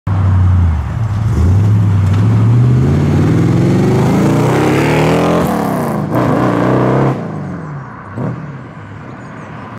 Rover V8 powered Capri Pulling away - RPi Engineering Exhaust Sound
Here's a little video of the Capri pulling away for the next stage of testing and tuning. It doesn't take long to reach the 40mph limit. Full video coming soon